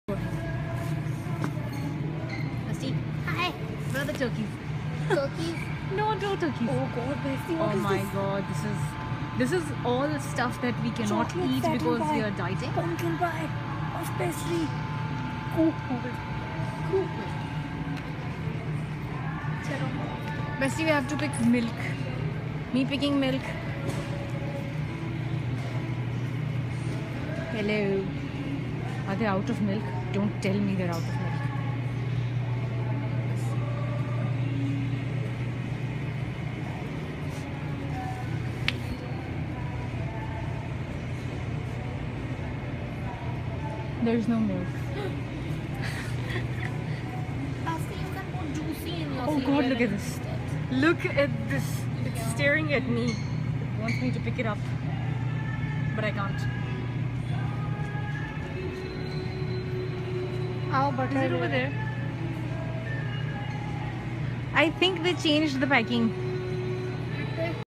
Shopping Shenanigans 001 Safeway - Buying Milk & Craving SBUX
Crazy shenanigans of me and Bestie shopping for milk at Safeway.